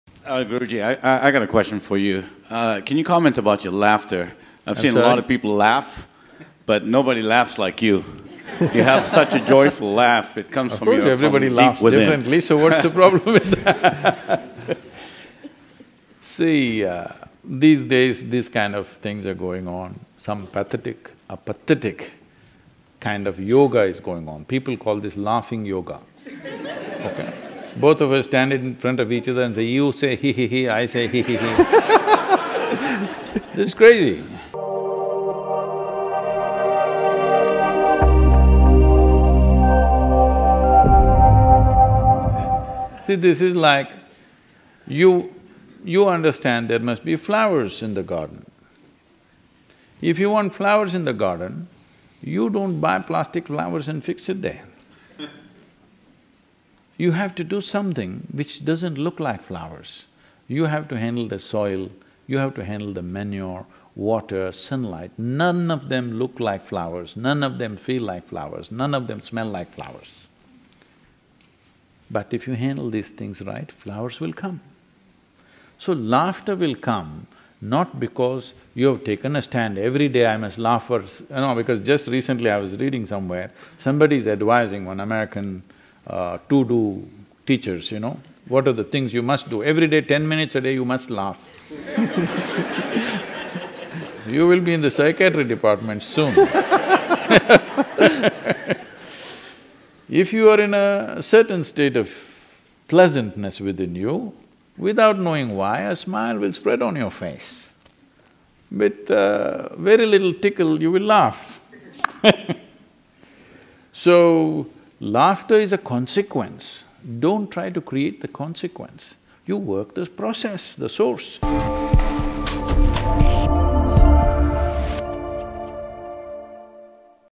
Sadhguru on Laughing Yoga
Sadhguru comments on Laughing Yoga.

All the contents in this video have been sourced from the PUBLIC DOMAIN.
Permission to re-use Sadhguru’s videos and also to monetize them